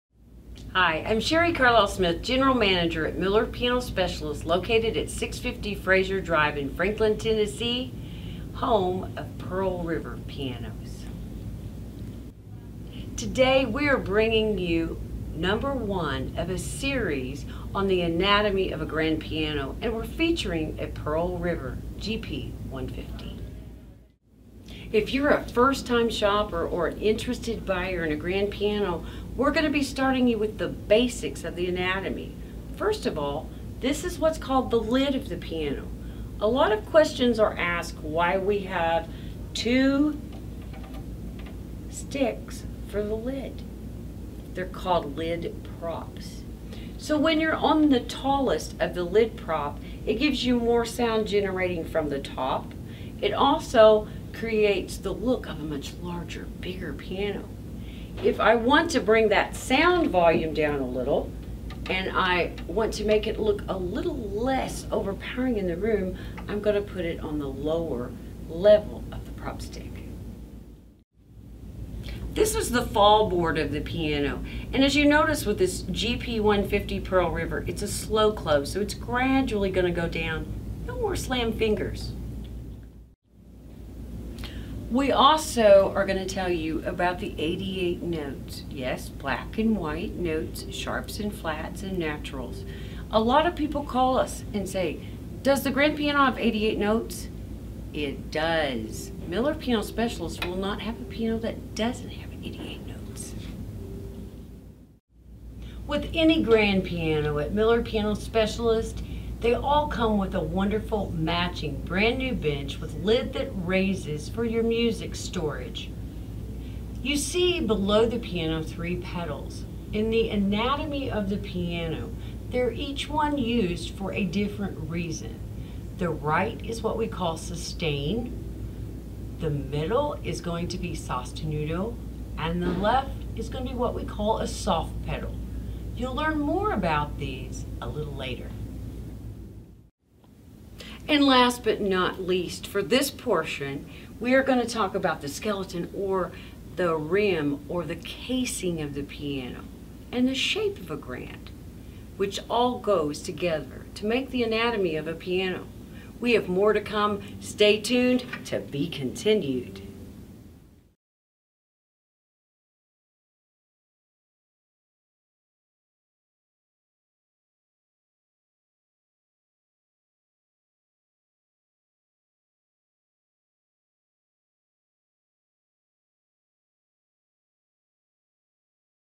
Anatomy of a Grand Piano Series Part One
This is a series of "Anatomy of a Grand Piano featuring a Pearl River GP 150 5' grand.  This series will take you from the basics - to the advanced working and craftmanship of a grand piano.  We are excited to educate those that are just starting and answer questions to those more advanced and "techy".